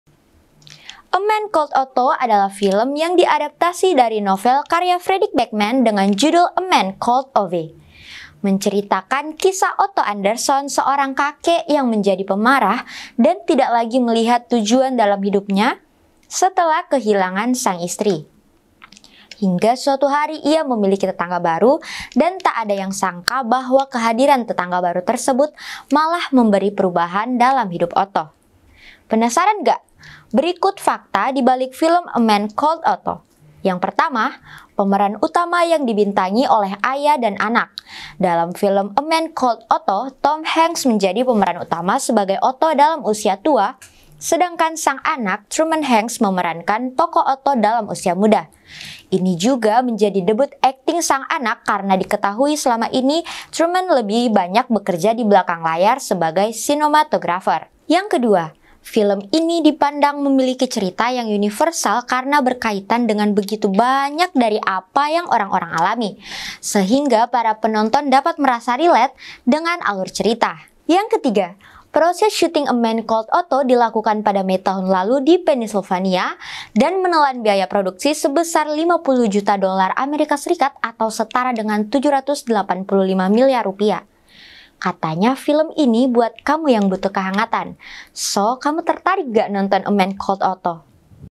Fakta Menarik Film A Man Called Otto, Film yang Memberi Kesan Kehangatan
A man called otto adalah film yg diadaptasi dari novel karya Fredik Backman dengan judul A Man Called Ove.
menceritakan kisah Otto Anderson seorang kakek yang menjadi pemarah dan tidak lagi melihat tujuan dalam hidupnya setelah kehilangan sang istri.
hingga suatu hari ia memiliki tetangga baru dan tak ada yang sangka bahwa kehadiran tetangga baru tersebut malah memberi perubahan dalam hidup Otto.
penasaran gak? berikut fakta dibalik film A man called otto

1. Pemeran utama yg dibintangi oleh ayah dan anak. Dalam film A man Called otto, Tom Hanks menjadi pemeran utama sebagai Otto dalam usia tua sedangkan sang anak Truman Hanks memerankan tokoh Otto dalam usia muda. ini juga menjadi debut akting sang anak, karena Diketahui, selama ini Truman lebih banyak bekerja di belakang layar sebagai sinematografer.
2.  Film Ini dipandang memiliki cerita yg universal karena berkaitan dengan begitu banyak dari apa yang orang-orang alami, , sehingga para penonton dapat merasa relate dengan alur cerita.

3. Proses syuting A Man Called Otto dilakukan pada Mei tahun lalu di Pennyslvania dan menelan biaya produksi sebesar 50 juta dolar AS atau setara dengan Rp. 785 miliar.

Katanya film ini buat kamu yang butuh kehangatan, so kamu tertarik gak nonton A man Called otto?